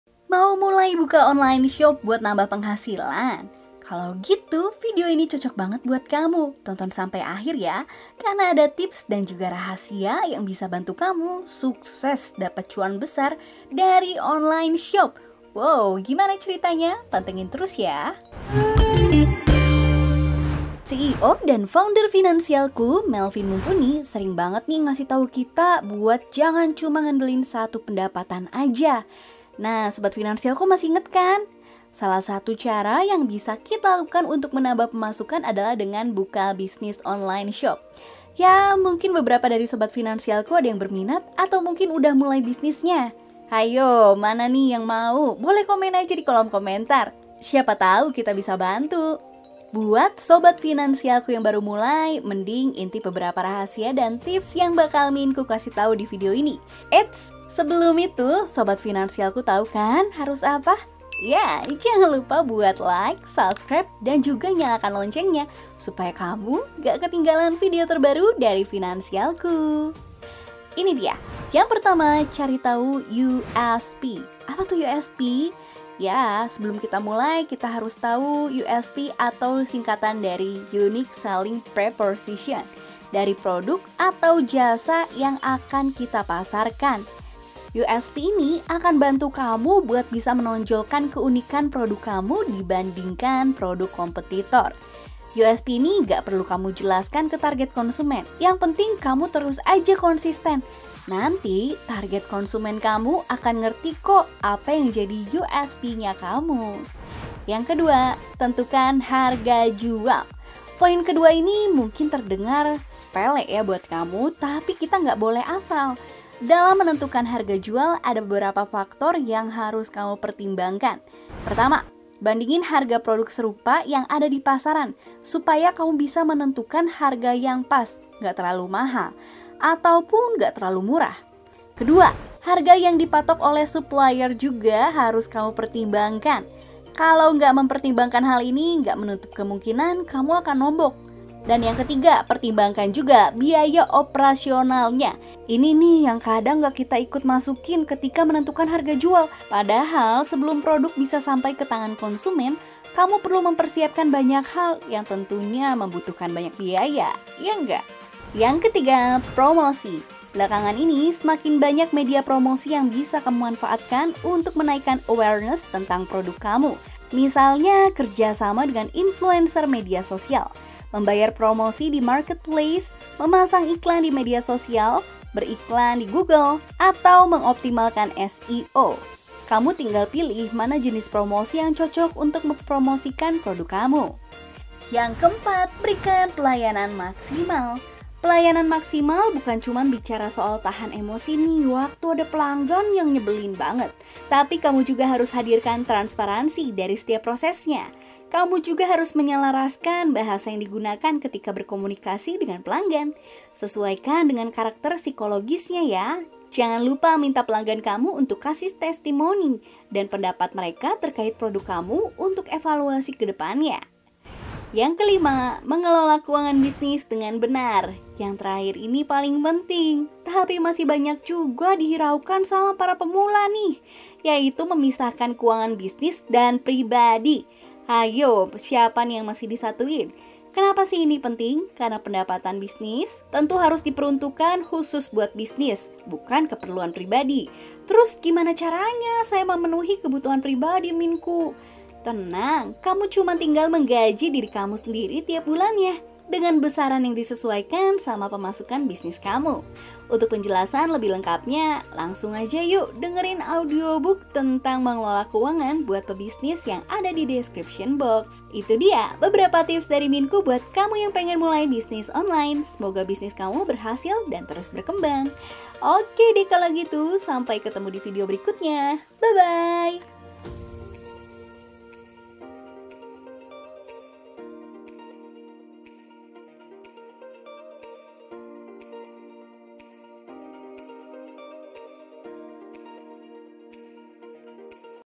Mudah Dilakukan!! Gini Cara Dapat Cuan Ratusan Persen Dari Bisnis Online!!
Yuk, yang mau nambah penghasilan! Gini nih caranya cuan ratusan persen dari bisnis online!! Udah coba belum??

Kamu mau mulai buka online shop buat nambah penghasilan? Kalau gitu, video ini cocok banget buat kamu tonton sampai akhir! Karena ada tips dan rahasia yang bisa bantu kesuksesan online shop kamu, lho!

CEO dan Founder Finansialku, Melvin Mumpuni sering banget kasih tahu kita untuk jangan cuma ngandelin satu pendapatan aja. Sobat Finansialku masih inget, ‘kan? Nah, salah satu cara yang bisa kita lakukan untuk nambah pemasukan adalah dengan buka bisnis online shop. Mungkin beberapa dari Sobat Finansialku ada yang berniat atau mungkin udah mulai bisnisnya, nih?

Buat Sobat Finansialku yang baru mulai, mending intip beberapa rahasia dan tips yang bakal Minku kasih tahu di video ini! Eits, sebelum itu, Sobat Finansialku tahu, ‘kan, harus apa? Yup!

1️⃣ Cari Tahu USP
Yang pertama, sebelum mulai, kamu harus tahu dulu apa USP dari produk atau jasa yang akan kamu pasarkan. USP sendiri adalah Unique Selling Proposition. Jadi, USP ini membantu kamu untuk bisa menonjolkan keunikan produk kamu dibandingkan produk kompetitormu. USP ini nggak perlu kamu jelaskan ke target konsumen, yang penting kamu terus konsisten, target konsumen akan mengerti dengan sendirinya, kok!

2️⃣ Menentukan Harga Jual
Poin kedua ini mungkin terdengar sepele buat kamu. Tapi, kita nggak boleh asal, lho! Dalam menentukan harga jual, ada beberapa faktor yang harus kamu pertimbangkan, yaitu:
🔴 Bandingkan harga produk serupa yang ada di pasaran, supaya kamu bisa menentukan harga yang pas, nggak terlalu mahal, atau pun nggak terlalu murah.
🔴 Harga yang dipatok oleh supplier juga harus kamu pertimbangkan, lho! Kalau nggak mempertimbangkan hal ini, nggak menutup kemungkinan, kamu akan nombok.
🔴 Pertimbangkan biaya operasional. Ini yang kadang nggak kita ikut masukkan ketka menentukan harga jual. Padahal, sebelum produk bisa sampai ke tangan konsumen, kamu perlu mempersiapkan banyak hal yang tentu membutuhkan biaya, ‘kan?

3️⃣ Promosi
Belakangan ini, semakin banyak media promosi yang bisa kamu manfaatkan untuk menaikkan awareness tentang produk kamu, misalnya kerja sama dengan influencer media sosial, membayar promosi di marketplace, memasang iklan di media sosial, beriklan di Google, atau mengoptimalkan SEO. Kamu tinggal pilih mana jenis promosi yang cocok untuk mempromosikan produk kamu.

4️⃣ Berikan Pelayanan Maksimal
Pelayanan maksimal bukan cuma bicara soal tahan emosi ketika ada pelanggan yang menyebalkan. Tapi, kamu juga harus hadirkan transparasi dari setiap prosesnnya. Kamu juga harus menyelaraskan bahasa yang digunakan ketika berkomunikasi dengan pelanggan. Sesuaikan dengan karakter psikologisnya, ya! Jangan lupa minta pelanggan kamu untuk kasih testimonial, dan pendapat mereka terkait produk kamu untuk evaluasi ke depannya, ya!

5️⃣ Mengelola Keuangan Bisnis dengan Benar
Yang terakhir ini paling penting, tapi masih banyak juga dihiraukan sama para pemula, nih, yaitu memisahkan keuangan bisnis dan pribadi. Kenapa ini penting? Karena pendapatan bisnis tentu harus diperuntukkan khusus buat bisnis, bukan keperluan pribadi. Terus gimana caranya saya memenuhi kebutuhan pribadi, Minku? Tenang, kamu cuma tinggal menggaji diri kamu sendiri setiap bulannya, dengan besaran yang disesuaikan sama pemasukan bisnis kamu. Untuk penjelasan lebih lengkapnya, kamu bisa dengarkan audiobook tentang mengelola keuangan buat pebisnis yang ada di description box, ya!